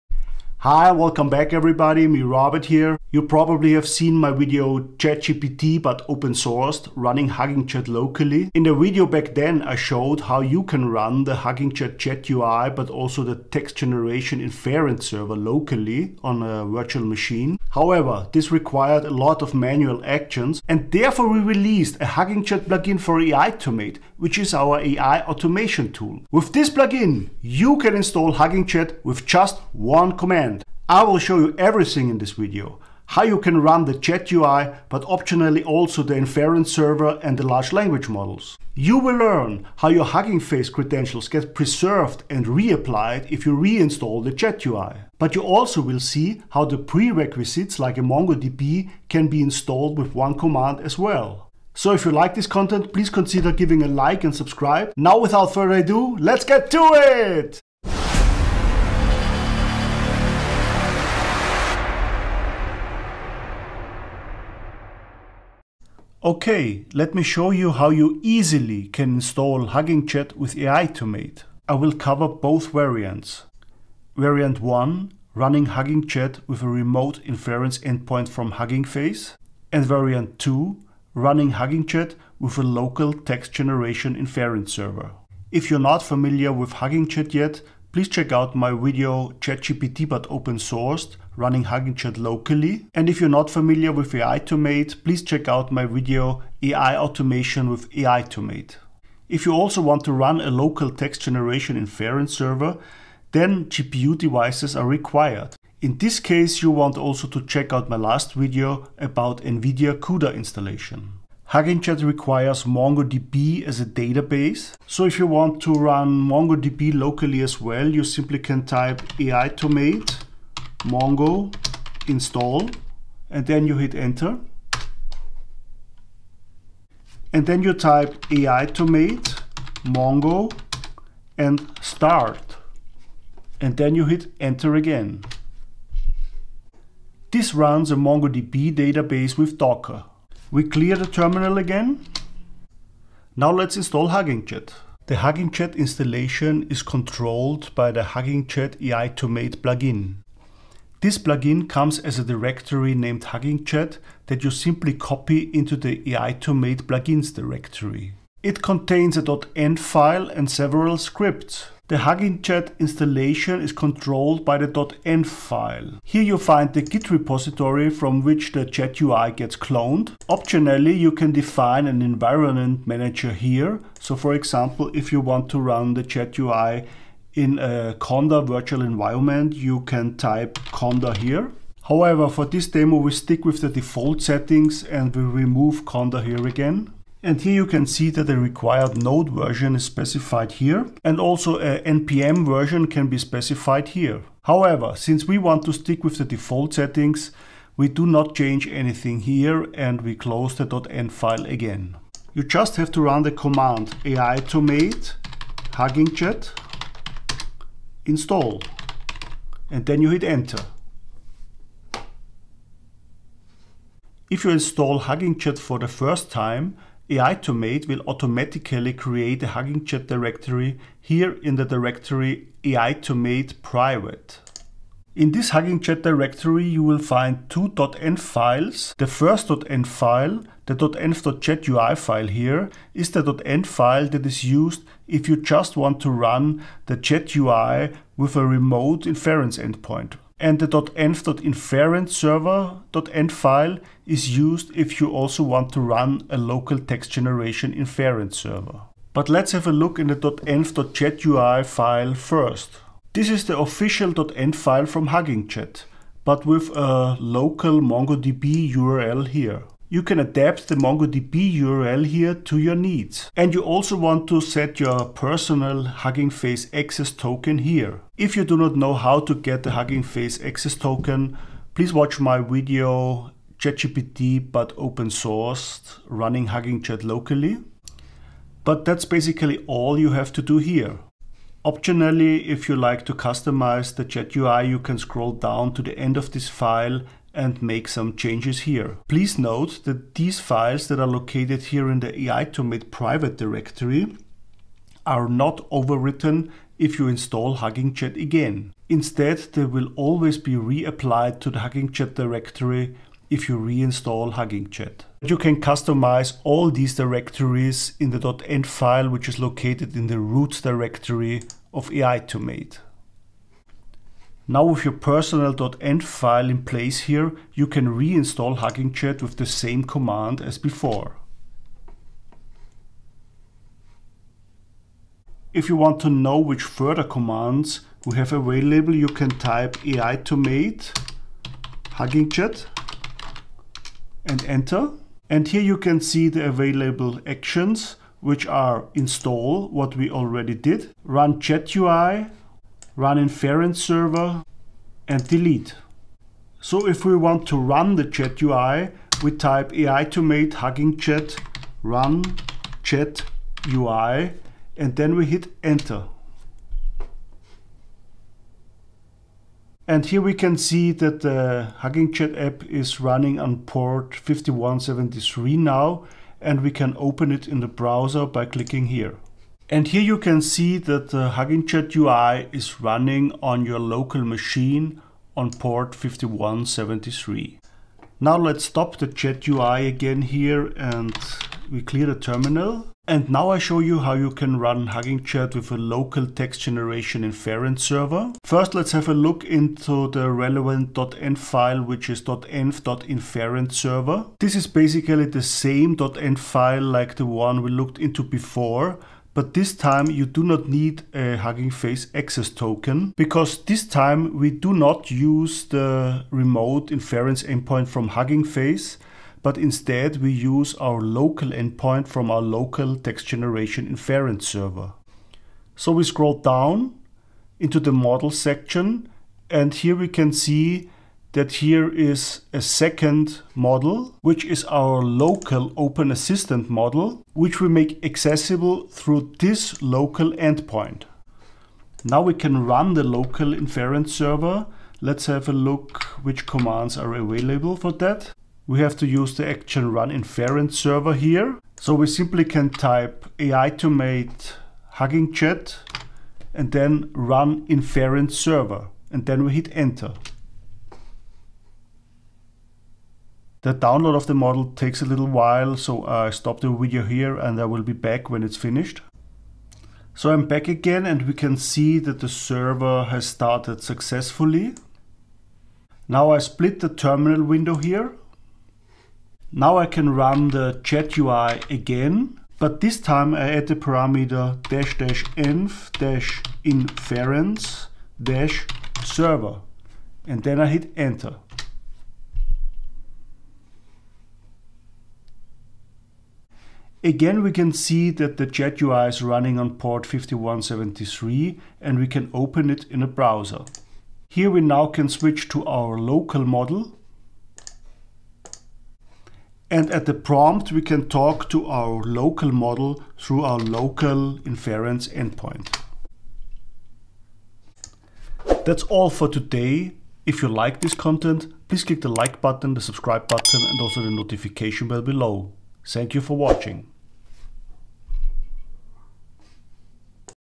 HuggingChat Installation with aitom8 Plugin | Chat-UI + Inference Server + LLM
In this video you learn how you can install HuggingChat with just one command. All you need is aitom8 and the HuggingChat aitom8 Plugin.
We cover everything, the Chat-UI, the Text Generation Inference Server and the Large Language Models (LLM). But also the prerequisites like a MongoDB.

Chapters in this video:
0:00 - Intro and Explanation
01:05 - Overview HuggingChat Installation
01:51 - MongoDB Installation
02:24 - HuggingChat aitom8 Plugin
03:22 - Install HuggingChat with one command
05:46 - Run the Chat-UI
06:23 - Install the Text Generation Inference Server
07:11 - Run the Text Generation Inference Server
08:28- Outro